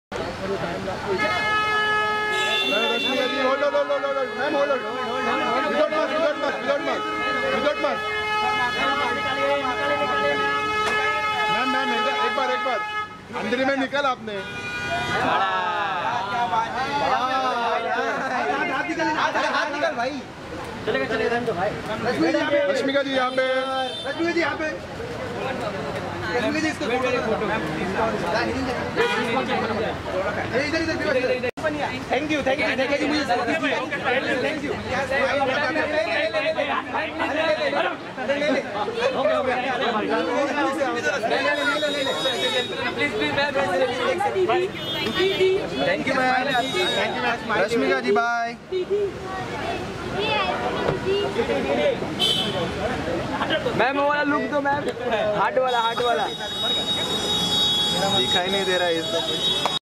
Rashmika Mandanna Spotted Tori Restaurant Outside At Bandra
Rashmika Mandanna